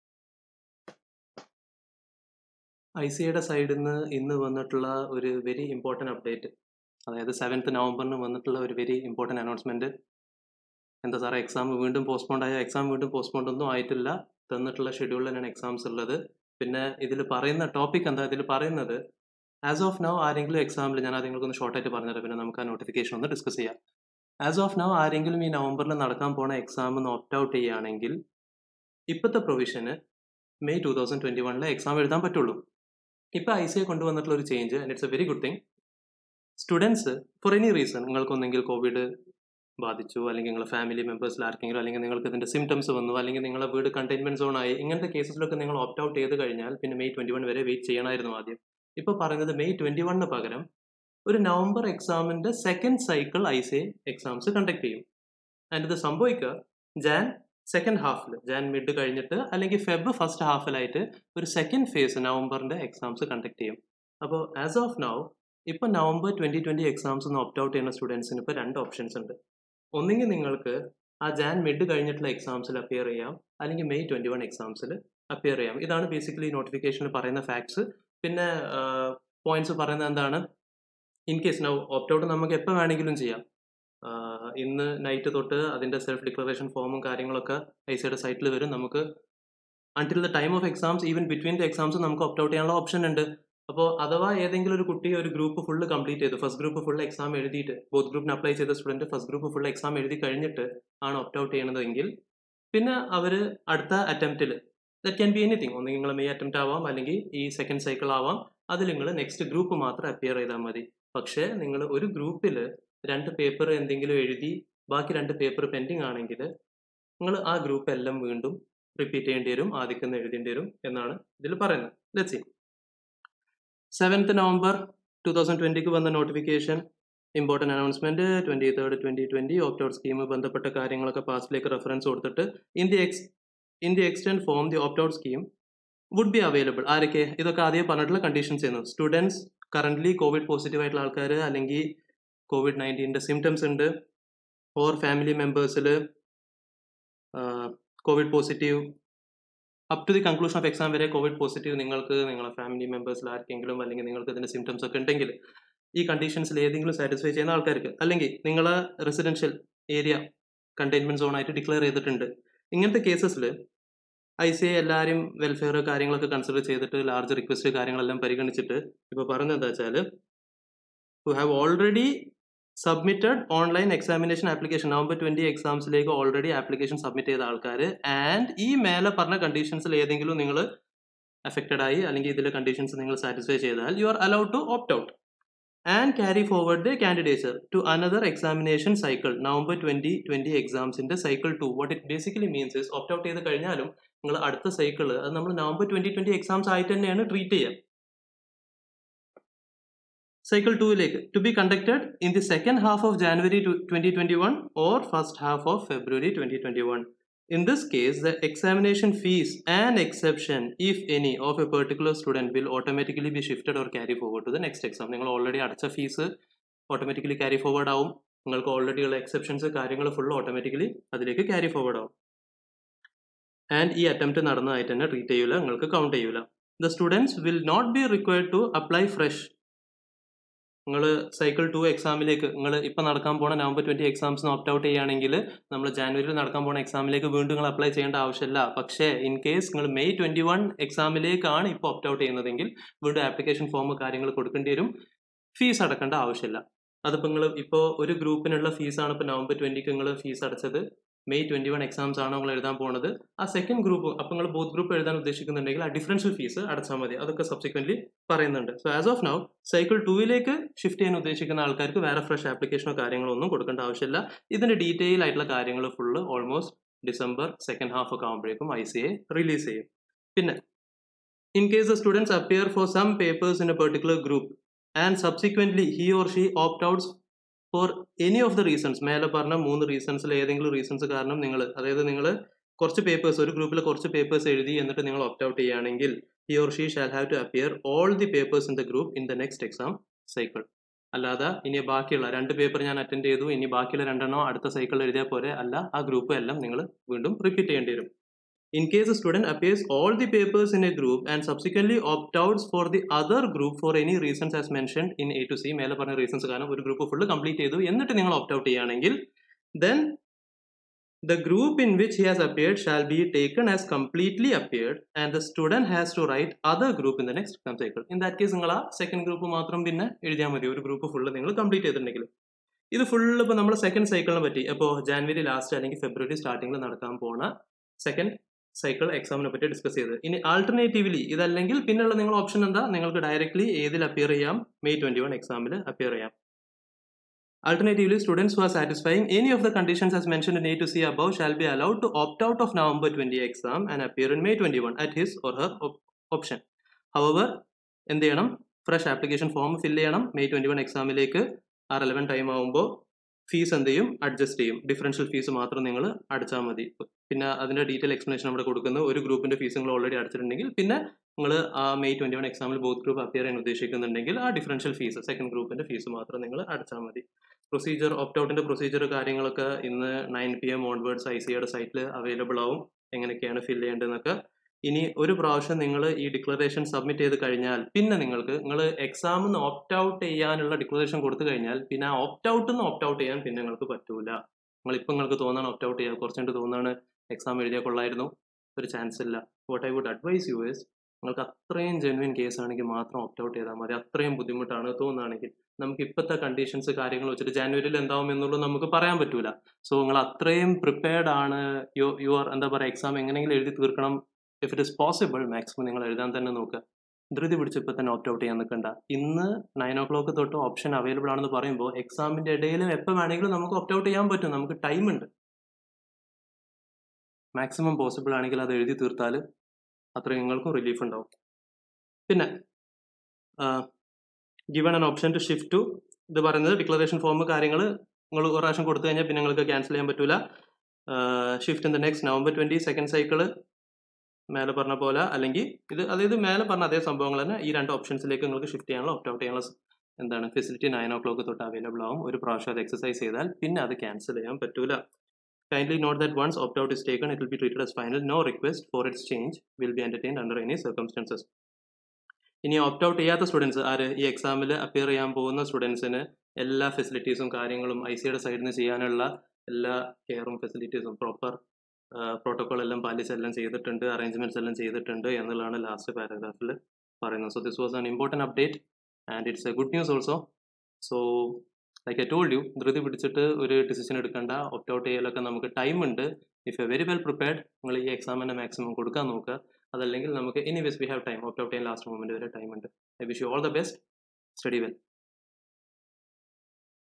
ICAI - Nov 2020 exams Cycle - II & Opt Out | 7.11.2020 | CA Shanil Usman | Malayalam
Important Announcement from ICAI regarding ICAI - Nov 2020 exams Cycle - II & Opt Out